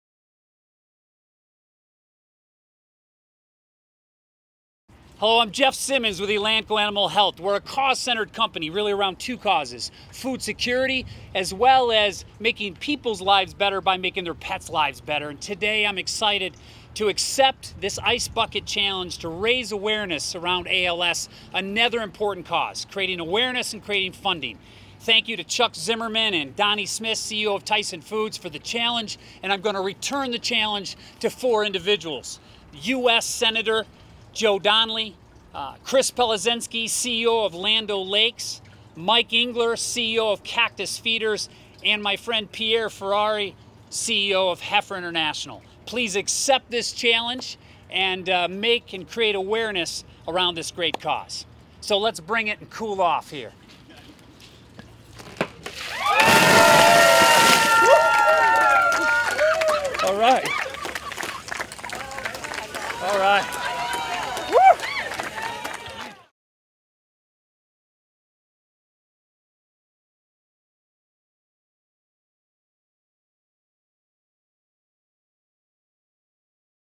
Jeff Simmons Accepts ALS Ice Bucket Challenge
Elanco president Jeff Simmons accepts the ALS Ice Bucket Challenge from Chuck Zimmerman and Tyson president and CEO Donnie Smith, then challenges U.S. Senator Joe Donnelly of Indiana, Heifer International CEO Pierre Ferrari, Land O'Lakes CEO Chris Policinski and Mike Engler, CEO of Cactus Feeders. In addition, a $100 donation will be made to the ALS Association.